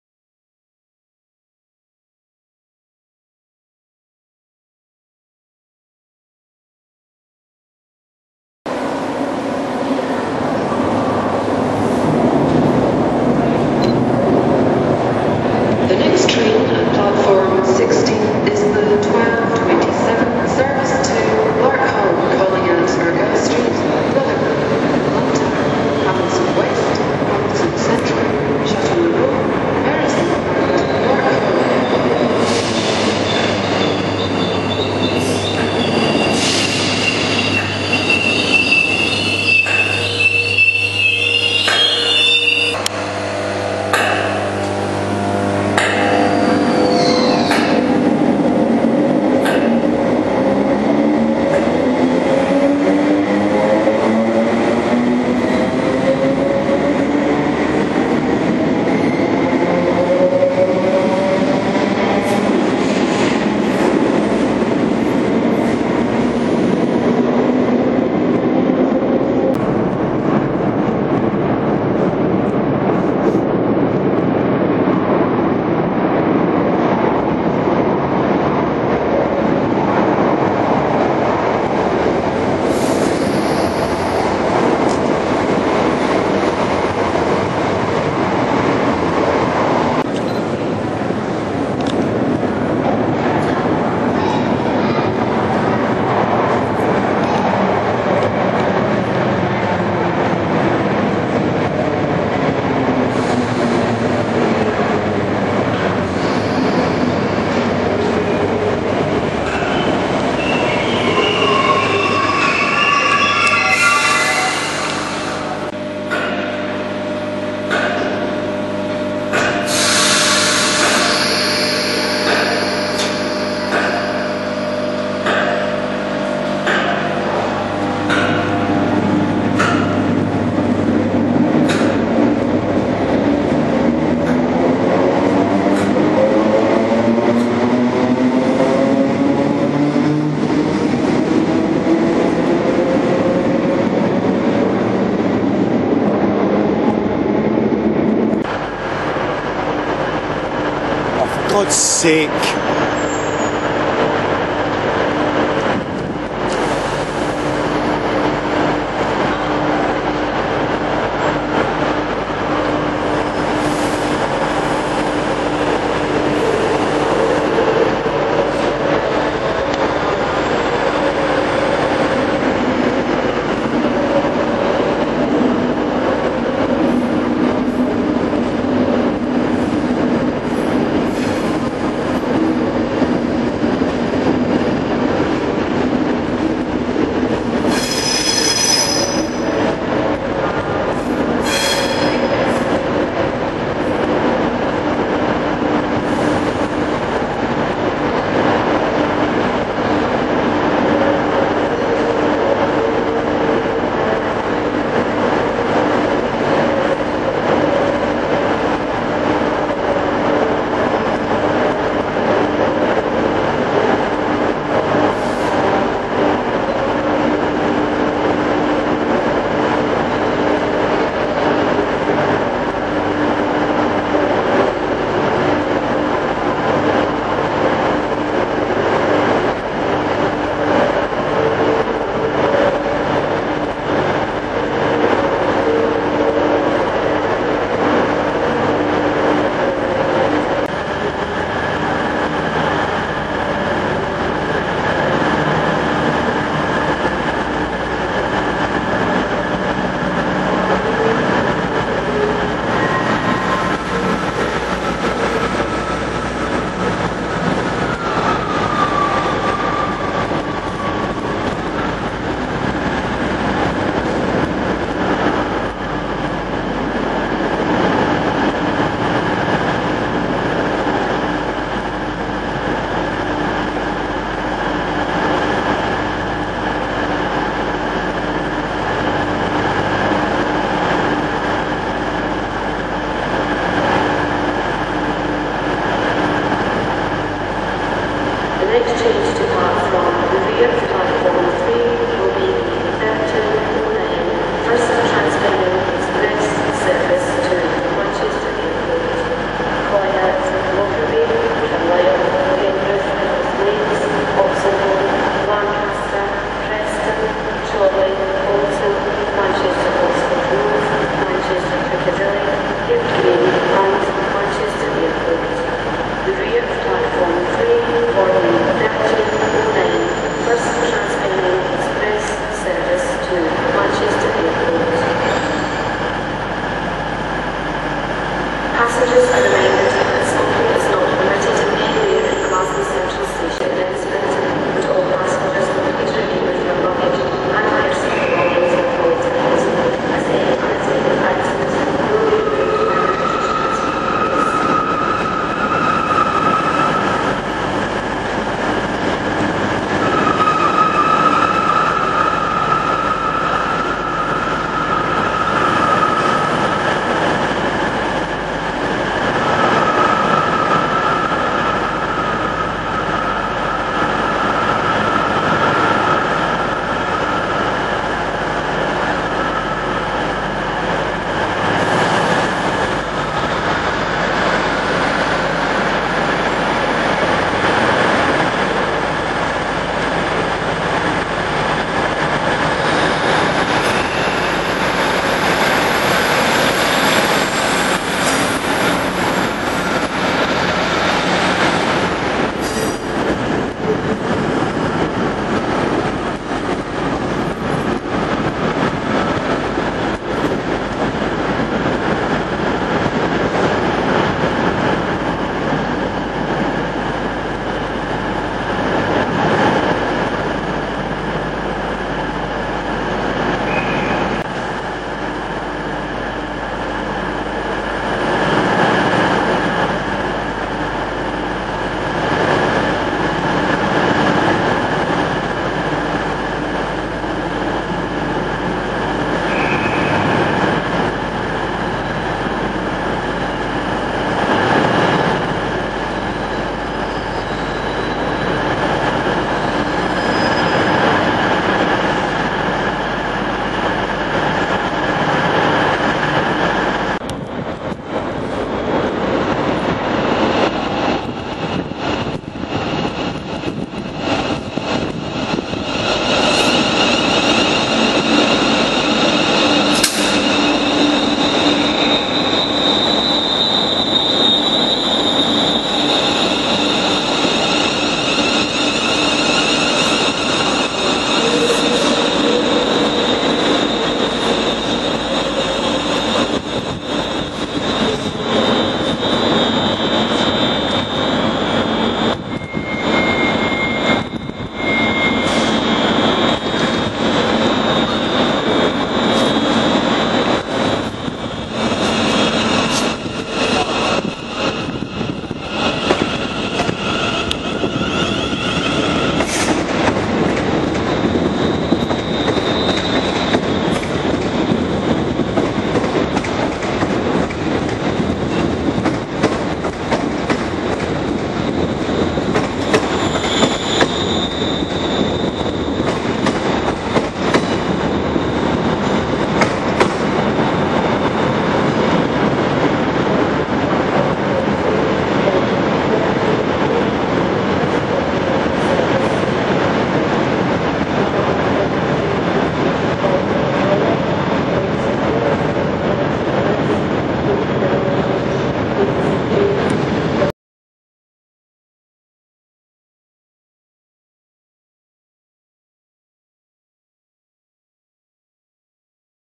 Glasgow Central - 18 February 2012
New territory for me, Glasgow Central, both low and upper level.